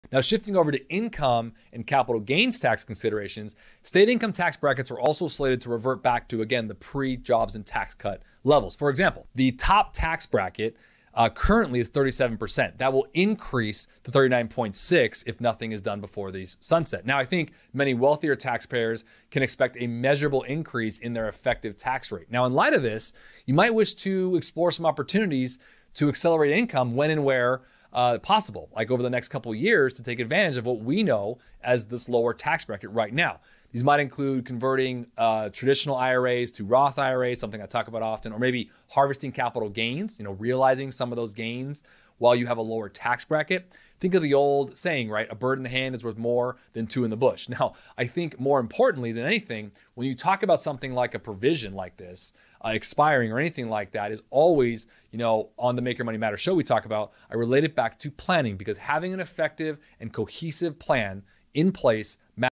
Tax Cuts Are Expiring
Tax Cuts Are Expiring: Convert your traditional IRA to a Roth IRA, realize capital gains while in lower tax bracket, and diversify your assets for your financial future.